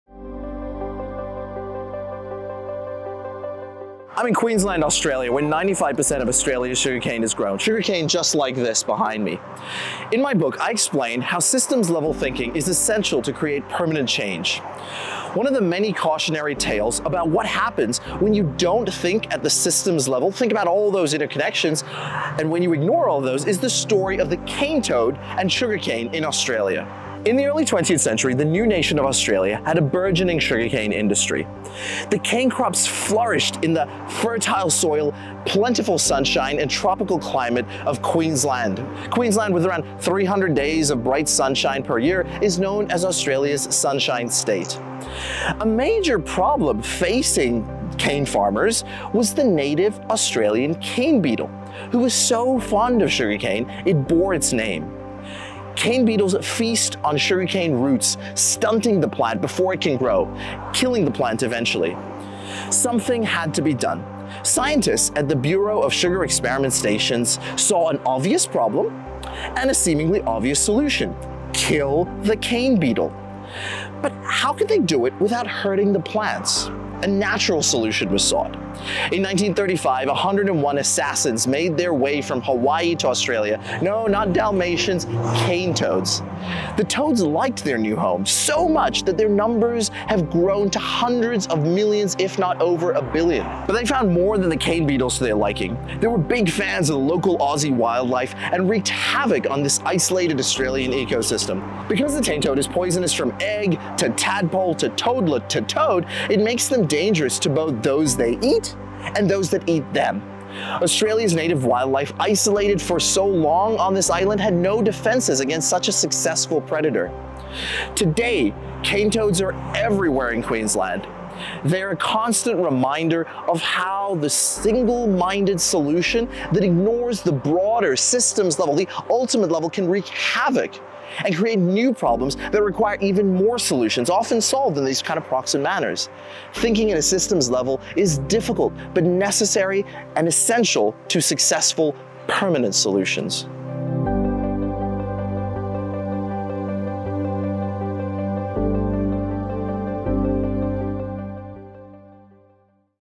Systems-level thinking is essential for permanent change: The Story of Australia's Cane Toads
Systems-level thinking is essential for permanent change. When you ignore complex interconnections, you create more problems. Like the cautionary tale of the cane toads and sugar cane in Australia.

Queensland’s 300 days of bright sunshine mean it's perfect for growing sugar cane, but farmers battled cane beetles who ruined their crops. Scientists thought for the obvious problem - cane beetles - there would need to be an obvious solution - kill the cane beetles. They wanted a natural solution, so introduced the cane toads.

But they didn't think of the broader ecosystem. The toads loved their new home, and their numbers grew to hundreds of millions. They wreaked havoc on the Australian ecosystem as they enjoyed consuming more than just the cane toad. Australia’s native wildlife had no defenses against the cane toad's, which is poisonous from eggs to tadpoles to toadlets to toads. Crocodile vs cane toads? Cane toads kill the croc even after they're dead.

Today, cane toads are present everywhere in Queensland and are a constant reminder of the failure of single-minded solutions. Cane toad solutions are everywhere in our societies, businesses, governments, and laws.